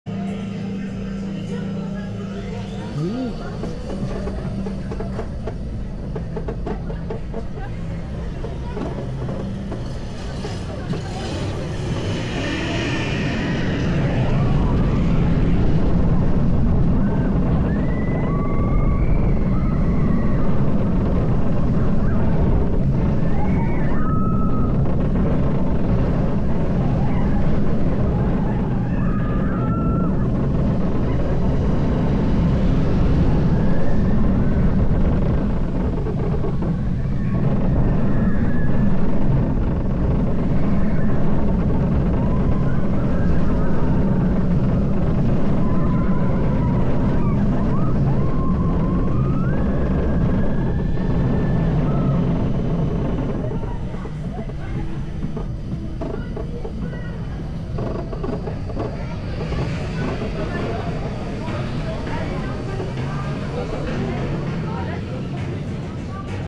Mahuka Ultrawide Front Row POV Onride 4K | First Single Rail Rollercoaster in Europe new 2024 Walibi
We rode Mahuka. Europes first single-rail rollercoaster. A new Intamin Design and model called Hot Racer. It is pure fun and gives you real jungle racer vibes. Watch our Onride and then visit the park. It's worth it!

This is a Ultra Wide POV Front Row onride captured with the Insta 360 X4